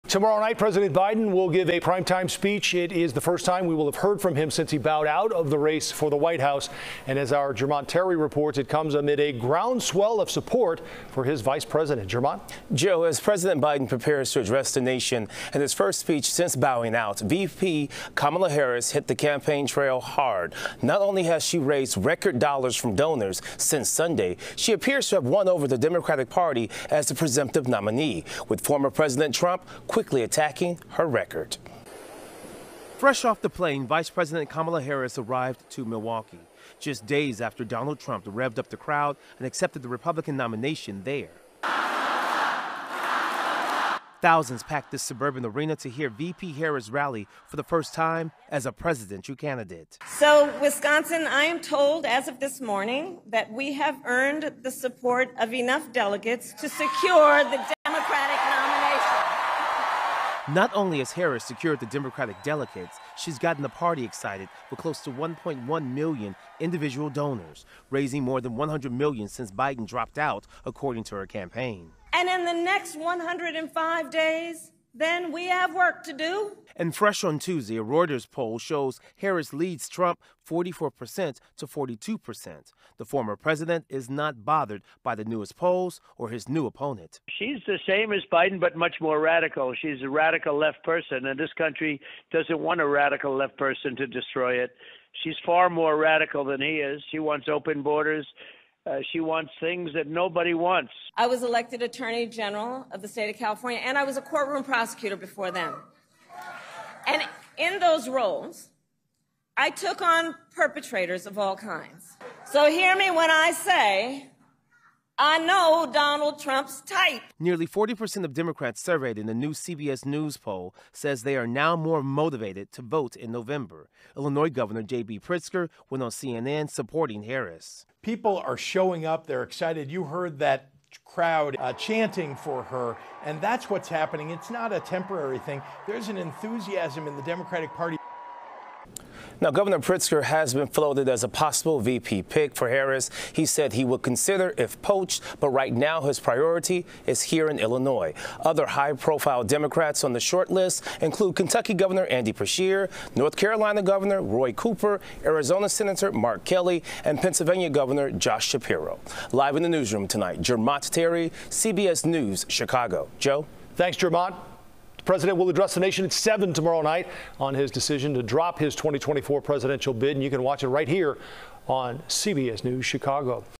Vice President Kamala Harris hits campaign trial hard as likely presidential nominee
Harris appears to have won over the Democratic Party as the presumptive nominee since President Biden withdrew, and former President Trump quickly attacked Harris' record. Jermont Terry reports.